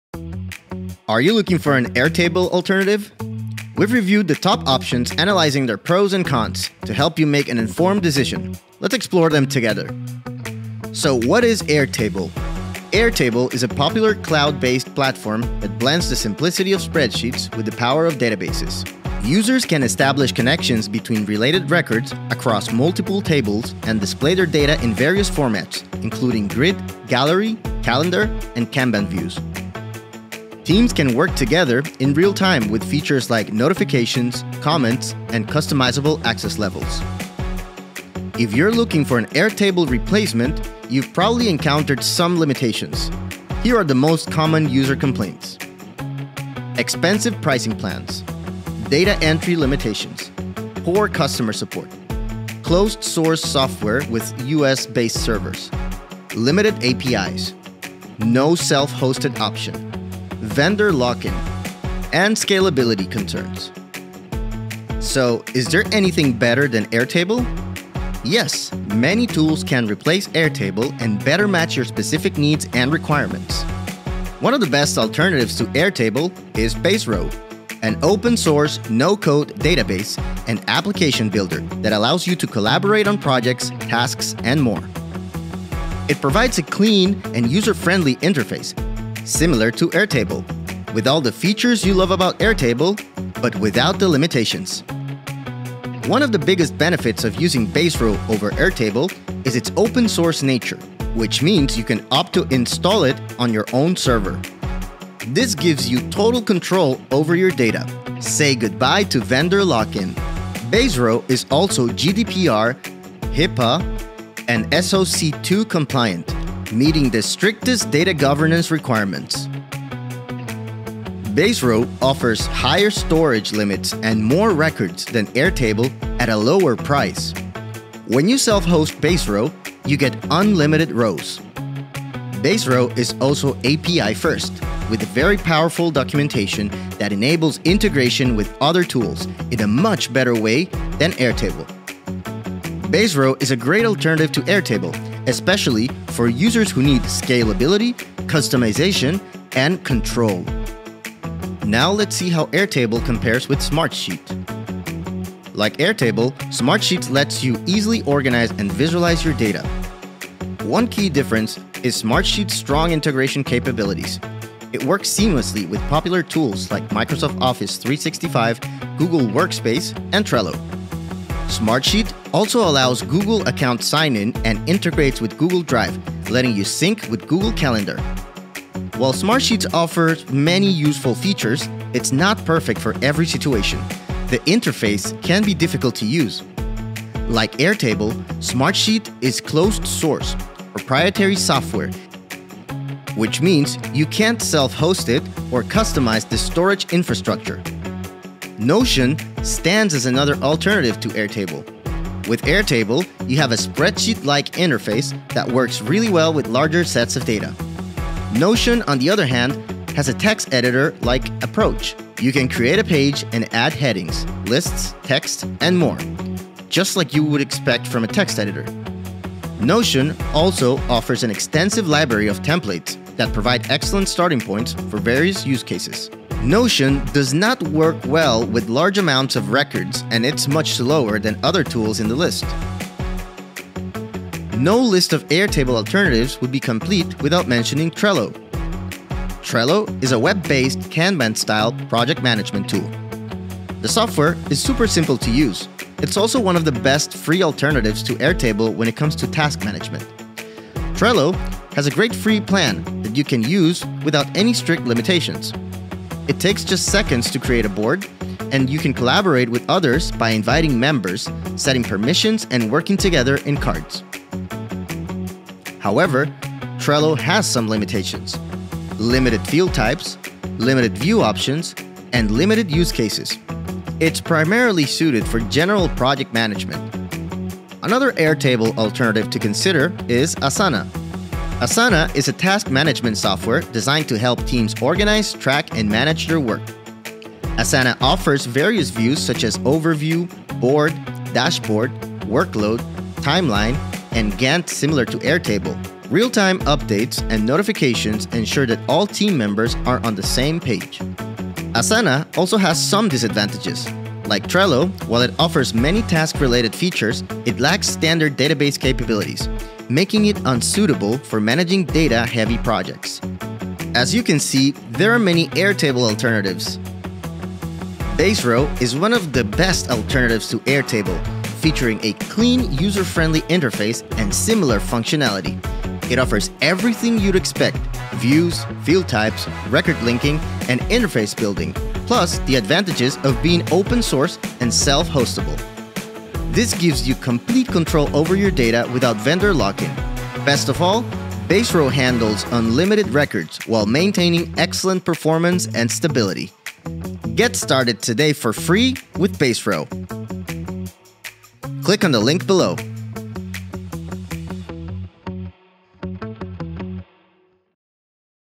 5 Best Airtable Alternatives in 2025 (No-Code & Open-Source Options!)
Looking for the best Airtable alternatives in 2025? Whether you're frustrated with Airtable's pricing, limitations, or lack of open-source flexibility, we’ve got you covered!

In this video, we compare Baserow, Smartsheet, Notion, Trello, and Asana—highlighting their key features, pros, cons, and best use cases for database management, project tracking, and team collaboration.

🔍 What you'll learn:

✅ The best no-code database and project management tools
✅ How Baserow compares to Airtable as an open-source alternative
✅ The pros and cons of cloud-based vs. self-hosted solutions
✅ Which tool fits your budget and scalability needs

📌 Timestamps:

00:00 Why Look for Airtable Alternatives? (Limitations & Pricing)
01:15 Baserow – The Best Open-Source Airtable Alternative 🚀
02:33 Smartsheet – Advanced Project Management
03:18 Notion – Notes & Docs Combined
03:54 Trello – Simple Kanban for Teams
04:38 Asana – Task & Workflow Management
05:14 Which Alternative is Best for You?